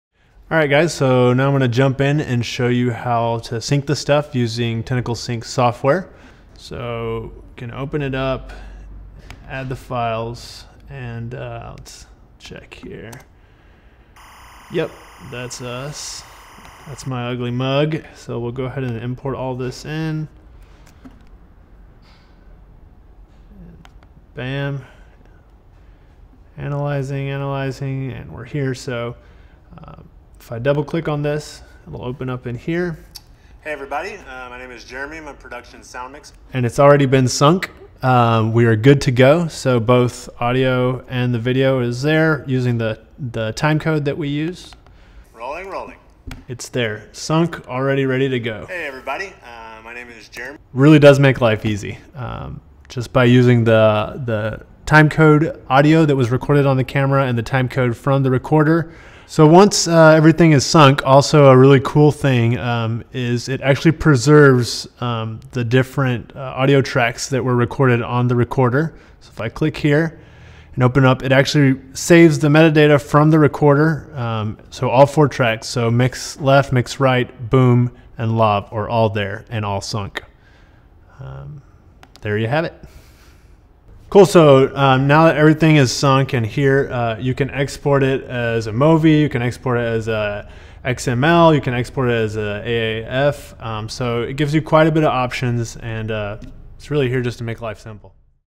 Hands On Tentacle Sync - Software - Part 2/2
This is the first hands on video showing the Sync Software. Thank you very much Jeremy for explaining the software.

Forget timecode – it’s the syncing that counts!
Tentacle Sync is a complete hardware/software solution that allows you to sync dozens of audio and video clips in seconds! It consists of 2 things:

- a super-small and user-friendly timecode generator (Tentacle)
- a powerful syncing software (Sync)

Tentacle is the ideal add-on for cameras like the Canon 5D or Blackmagic and enables them to effortlessly use timecode for syncing.